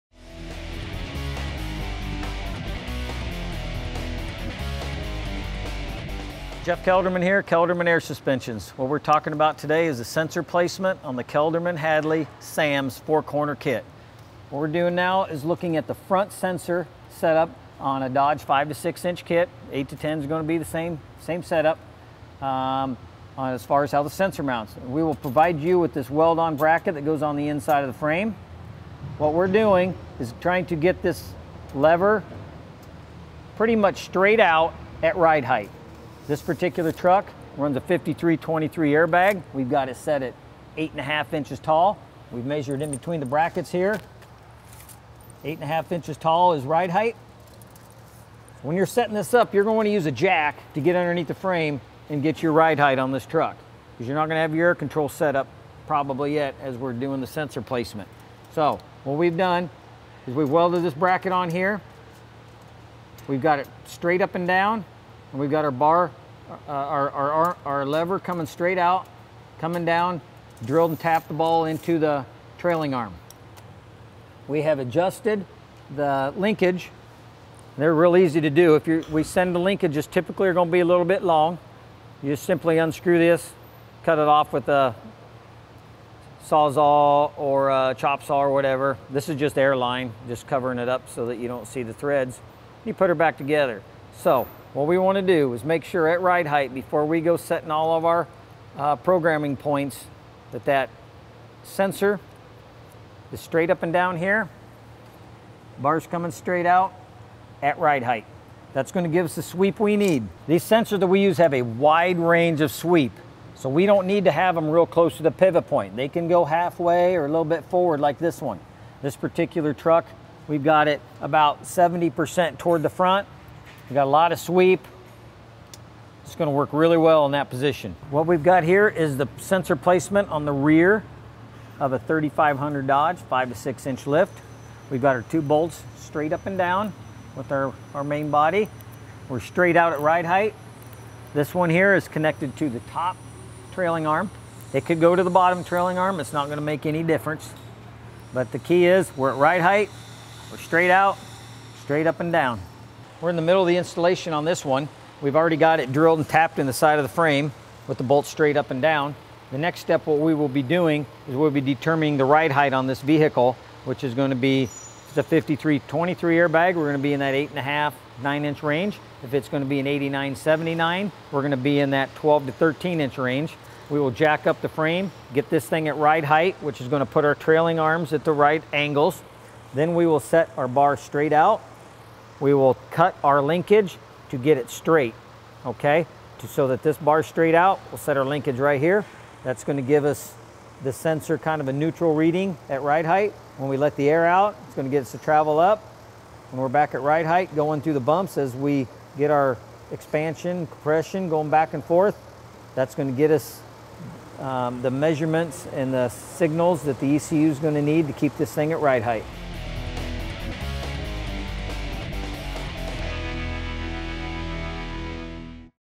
Kelderman Hadley SAMS sensor installation
Instructional video showing Hadley sensor placement on a Kelderman 5-6" Lift Kit.